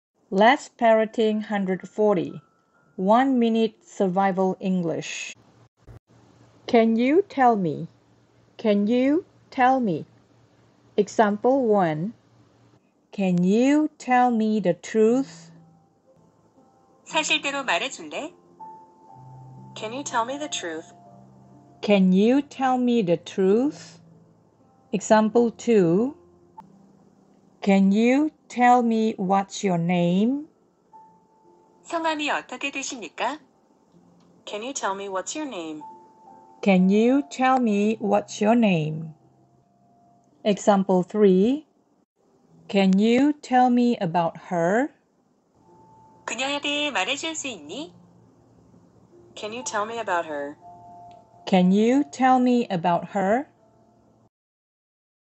Let's Parroting 140: One Minute Survival English Expression. 1분 생존영어 따라하기
Let's Parroting 140:
One Minute Survival English Expression :
Can you tell me~
~을 말해 줄 수 있어요?
따라해보세🍀

여러분 흔히 실수 또는 흔히 헷갈리는 영어표현들 좀 모여드려봤읍니다.
조금 더움 됬으면 합니다.
재미있게 연습하시길 바랍니다^^

이 Channel 맘에들시면
"좋아요" & "구독" 해주세요^.*
저에게 큰 힘이됩니다♡
귀한시간들 내주셨어 감사합니다.
늘 행복하세요🍀💌